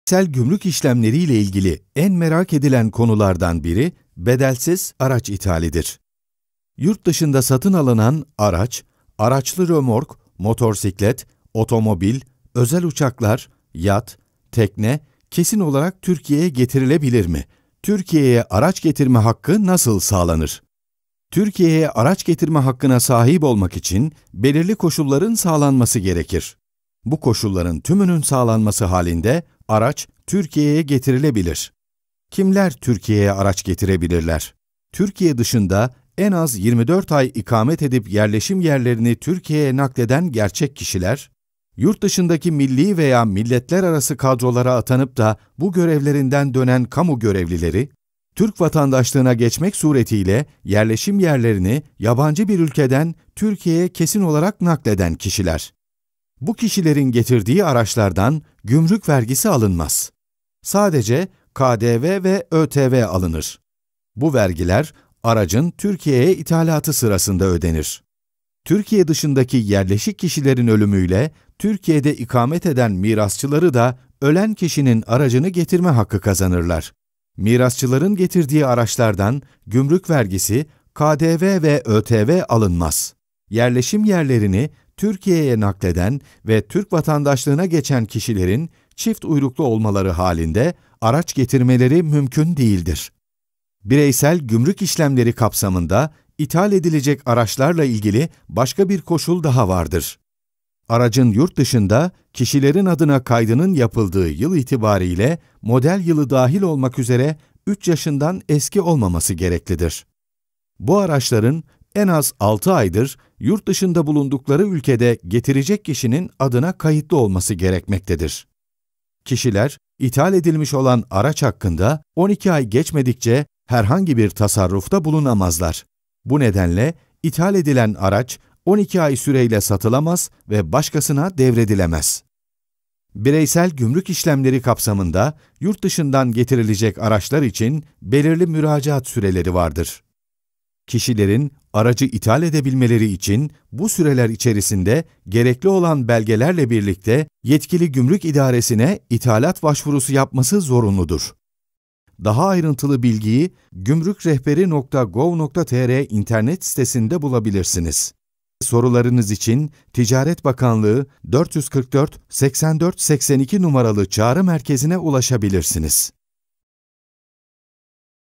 Türkiye’ye Nasıl Vergisiz Araba Götürülür / BEDELSİZ TAŞIT İTHALATI
Yurt dışında satın aldığınız aracınızı (araçlı romörk, motorsiklet, otomobil, özel uçaklar, yat, tekne) kesin olarak Türkiye’ye getirebilir misiniz? Türkiye’ye araç getirme hakkı nasıl kazanılır?